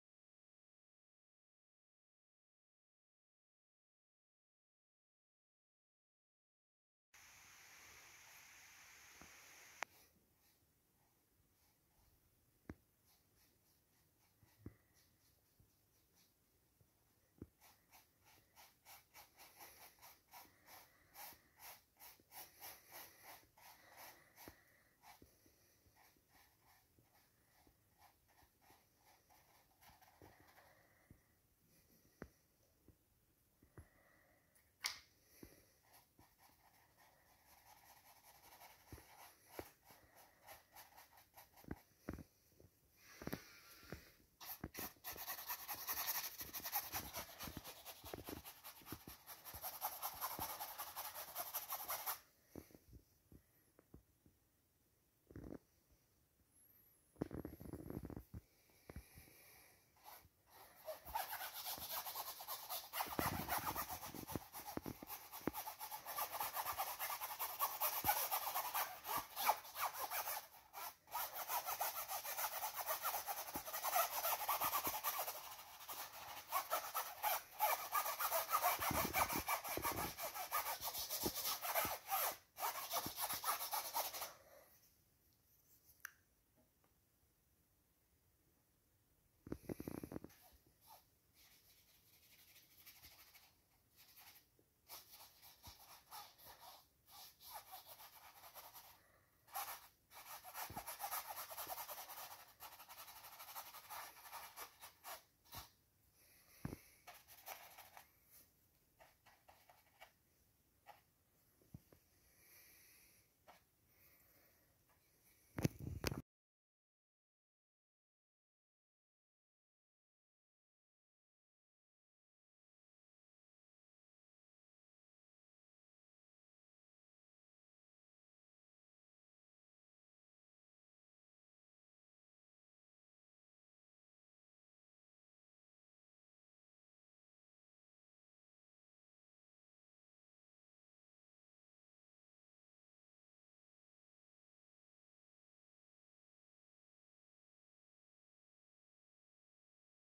Aquarell Lehr Video Auftragetechnik ohne Kommentar
Aquarell Farben alleine sind meiner Meinung nicht sehr ausdrucksstark. Aber bei Acryl Leinwänden eignen sie sich sehr gut um schnell leuchtende und schimmernde Hintergründe zu erzeugen.